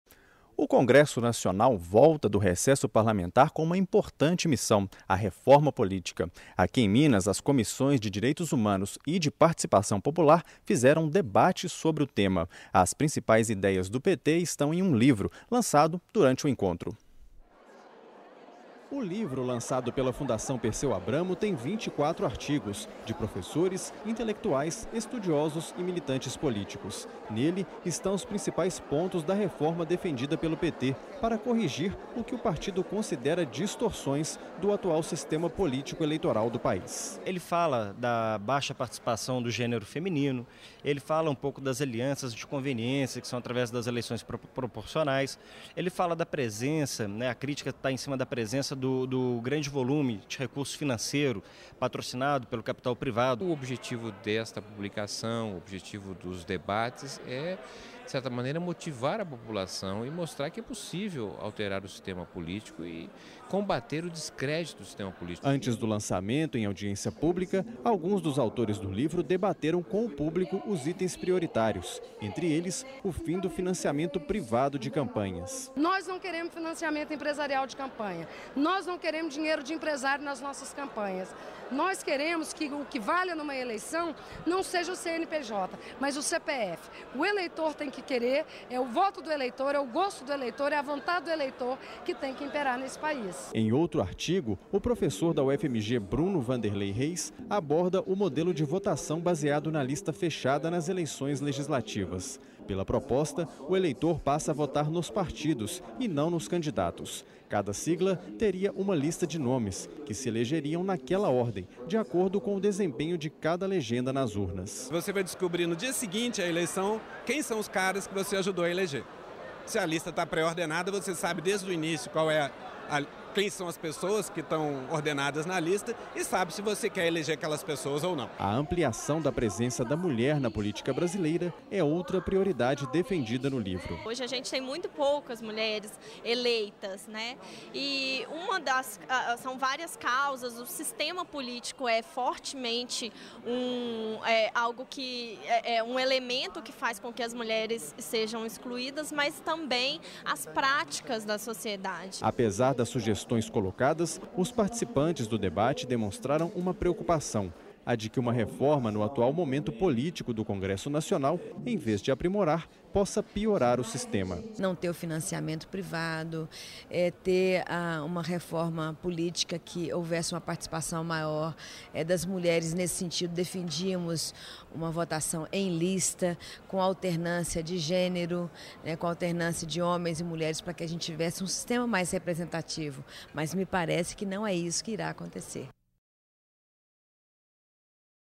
Participação popular deve nortear Reforma Política
As Comissões de Direitos Humanos e de Participação Popular promoveram debate sobre este tema. No evento de 3/8/2015, foi lançado o livro "Reforma Política Democrática - temas, atores e desafios”, editado pela Fundação Perseu Abramo em parceria com o PT nacional.